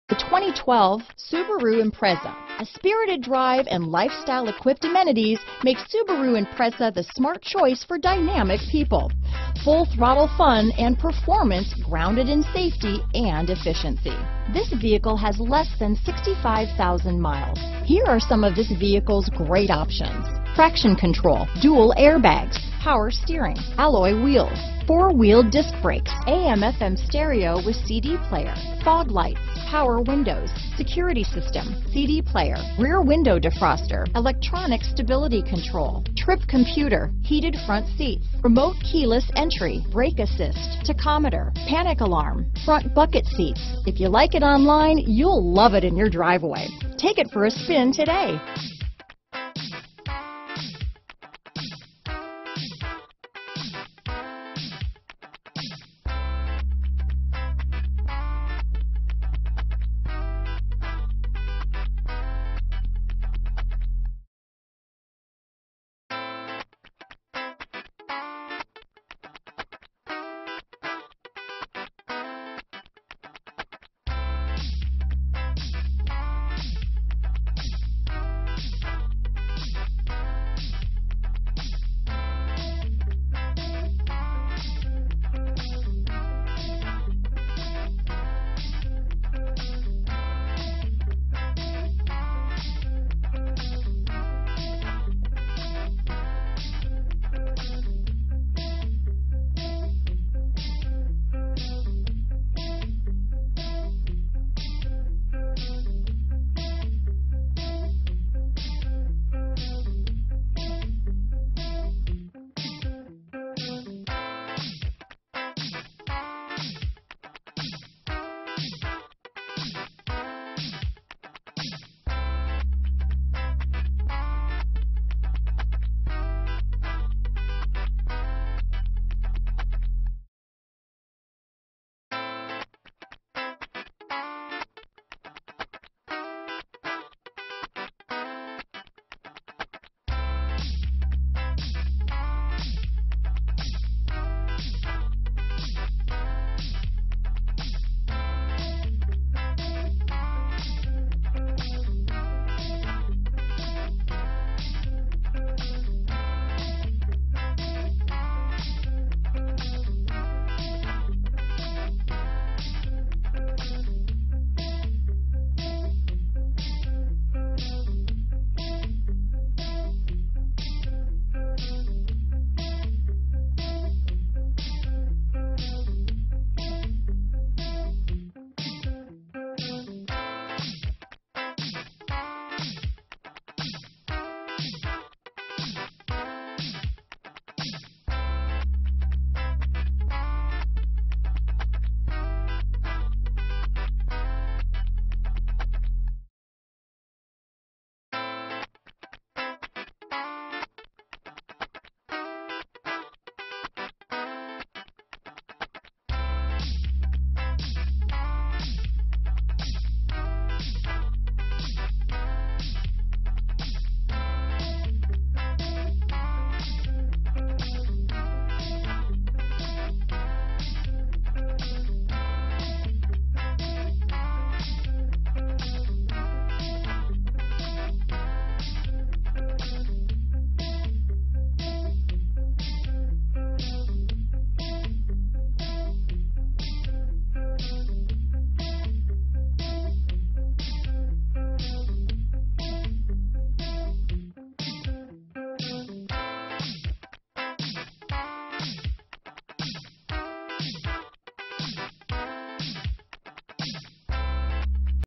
2012 Subaru Impreza Reno, Sparks, Lake Tahoe, Mammoth, Northern Nevada S18041A
2012 Subaru Impreza

AWD! Rally Race Inspired! There is no better time than now to buy this superb-looking 2012 Subaru Impreza. Whether it's navigating the jammed roadways or the crowded parking lots, you'll enjoy the surprising maneuverability of this car. This vehicle comes with a 90 day or 3,000 mile limiited powertrain warranty!!  This vehicle is NOT sold as-is!!!